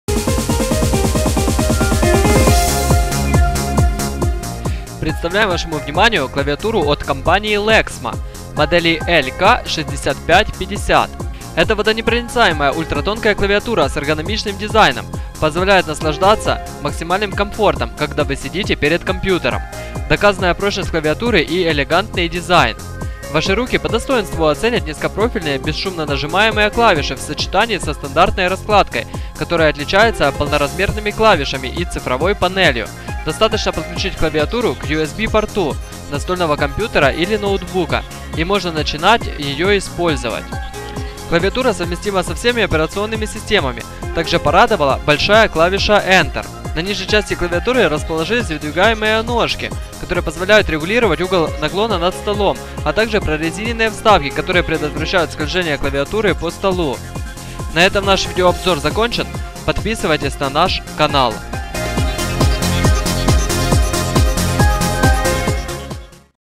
Lexma LK-6550 обзор / review
Водонепроницаемая  ултратонкая клавиатура LEXMA LK6550 USB с эргономичным дизайном позволяет наслаждаться  максимальным комфортом, когда вы сидите перед компьютером. Доказанная прочность клавиатуры и элегантный дизайн, количество нажатий клавиш этой профессиональной клавиатуры составляет более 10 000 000 раз.

Ваши руки по достоинству оценят низкопрофильные бесшумно нажимаемые клавиши в сочетании со стандартной раскладкой, которая отличается полноразмерными F-клавишами и цифровой панелью.

Достаточно подключить клавиатуру к USB-порту настольного компьютера или ноутбука, и можно начинать ее использовать.